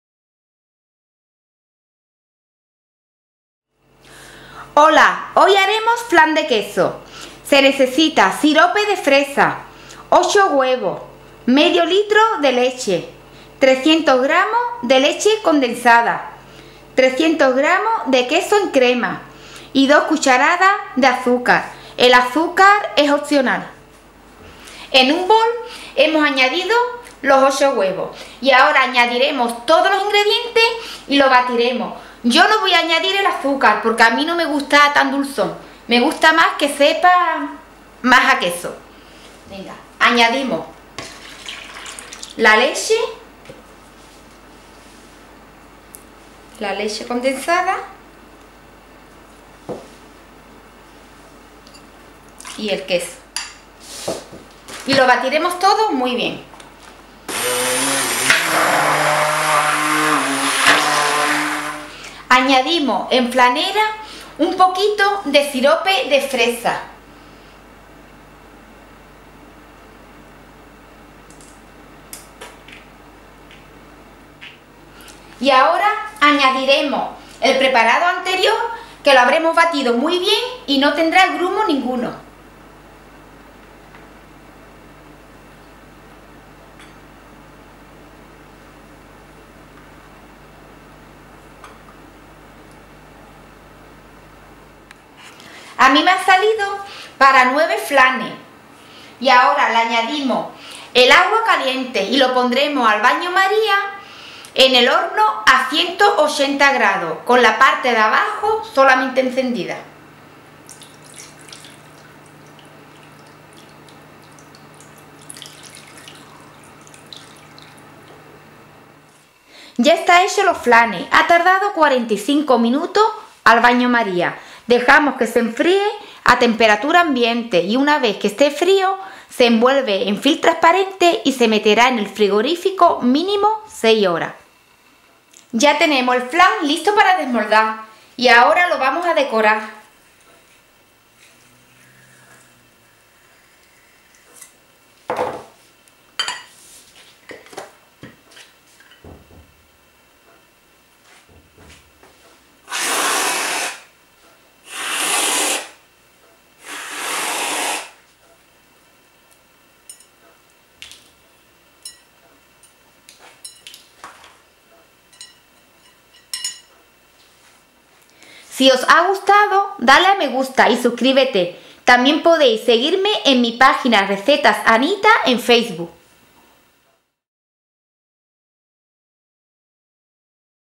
COMO HACER UN DELICIOSO FLAN DE QUESO,Facil de Hacer🍮
INGREDIENTES:
8 huevos
Medio litro de leche
300gr.Leche condensada
300gr.Queso crema
2 Cucharadas de azúcar (opcional)
Sirope de fresa